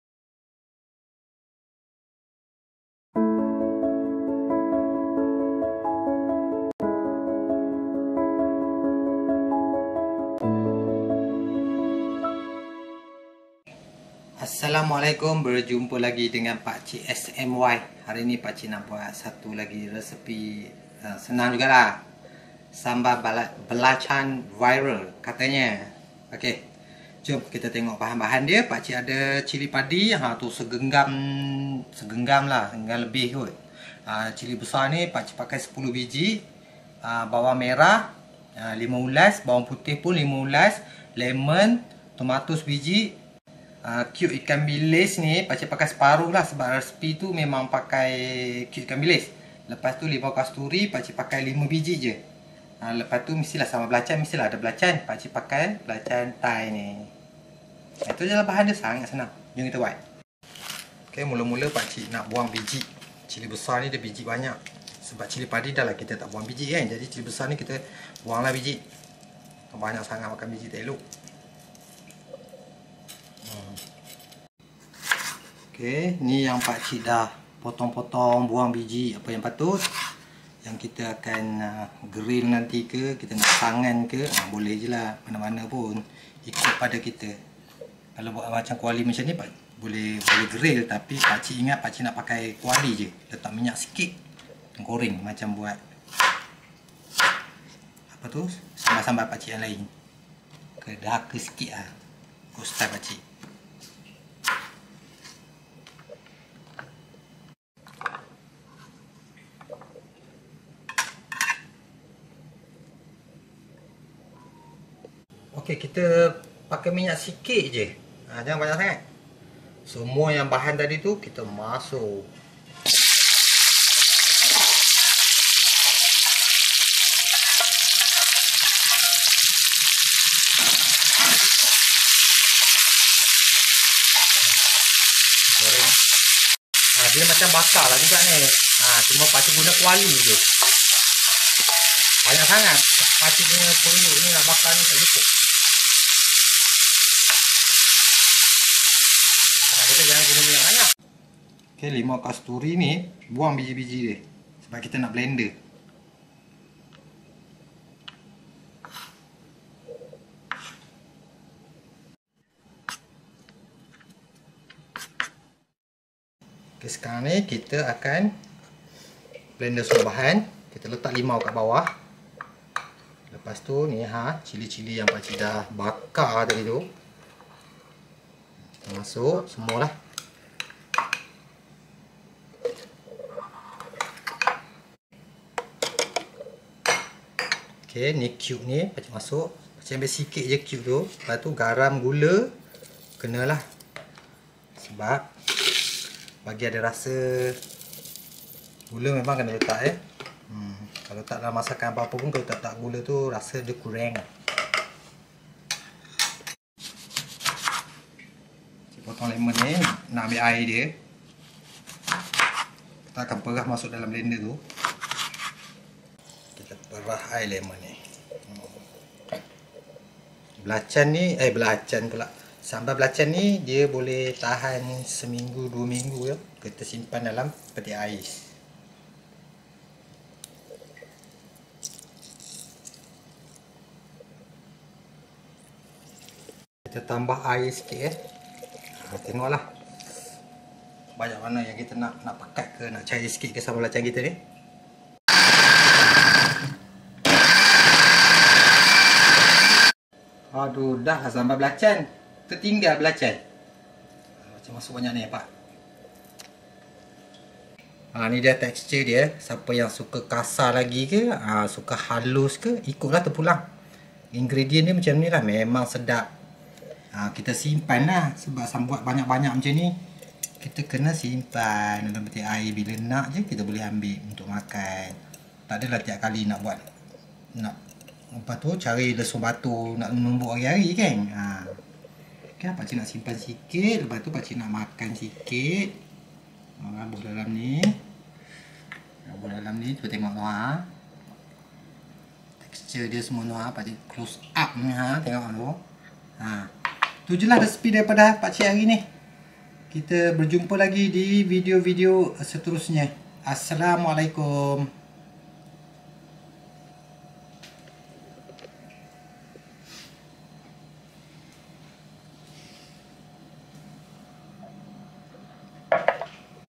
Resepi Sambal Belacan Viral Katanya!!! Ternyata memang sedap dan tahan lama
cara buat sambal belacan sedap
cara mudah buat sambal belacan

Pakcik kongsikan satu resepi sambal belacan yang sangat sedap pada tekak pakcik. Semuga perkongsian ini bermanafaat untuk kita semua. Selamat menonton dan terima kasih kerana terus menyokong channel pakcik smy.

Bahan-Bahan / Ingredients:-
- Cili Padi / Bird eye chillies
- Cili besar masak / red chillies
- Bawang merah kecil / Red onions
- Limau Kasturi / Calamansi limes
- Lemon / lemon
- Tomato / tomato
- Kiub Ikan bilis / Anchovies cube (knorr)
- Belacan / Shrimp paste

PREPARATION PLEASE WATCH THE VIDEO
SELAMAT MENCUBA

FOLLOW PAKCIK SMY